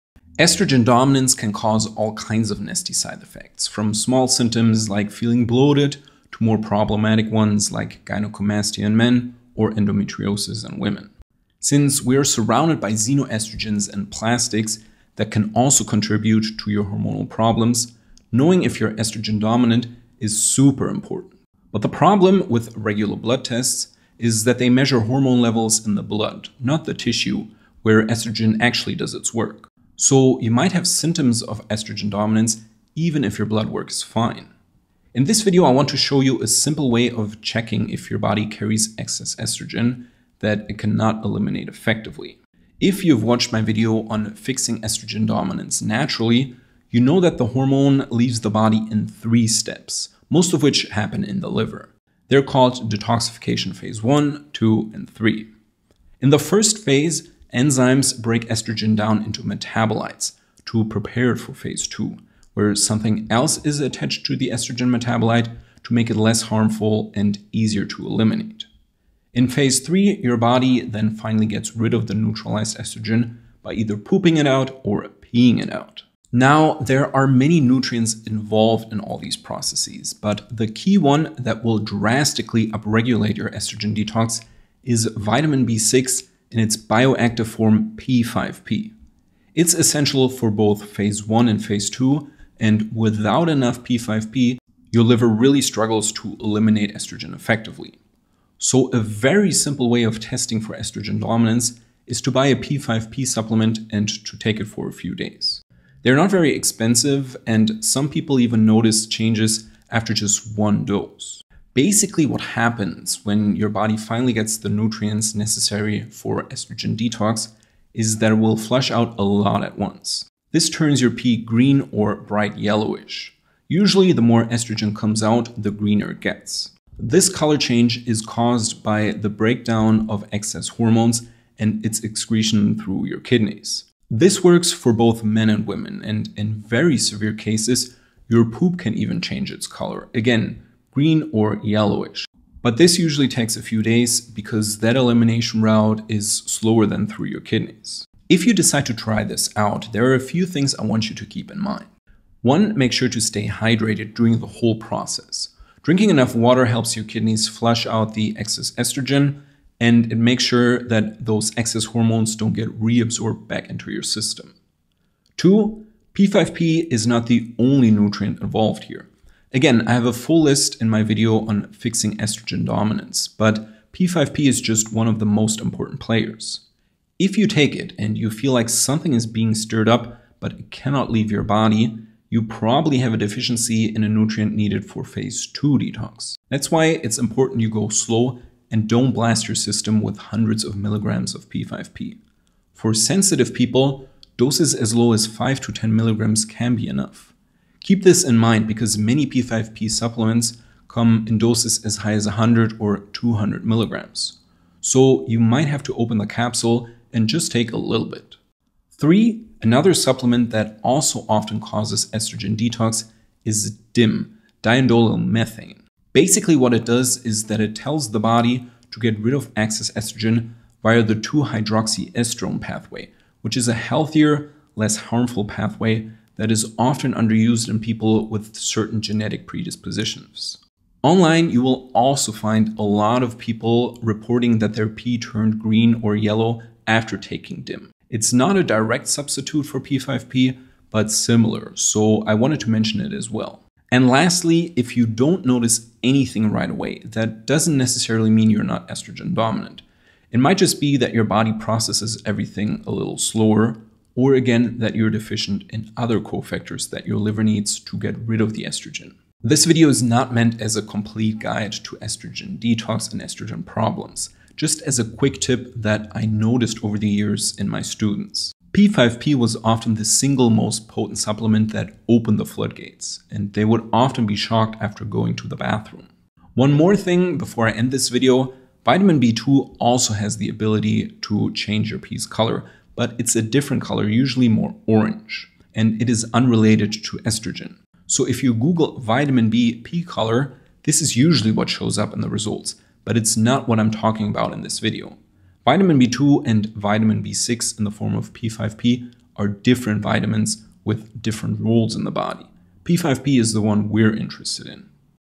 The Easiest DIY Test For Estrogen Dominance
Estrogen dominance can cause all kinds of nasty side effects from small symptoms like feeling bloated to more problematic ones like gynocomastea in men or endometriosis in women. In this video I want to show you a simple way of checking if your body carries excess estrogen that it cannot eliminate effectively

- Timestamps -
00:00 Introduction
00:52 How Your Liver Detoxes Estrogen
01:16 The Vitamin B6 (P5P) Test
02:32 Tips For Estrogen Test
03:28 DIM & Estrogen
04:36 Vitamin B2 & Pee Color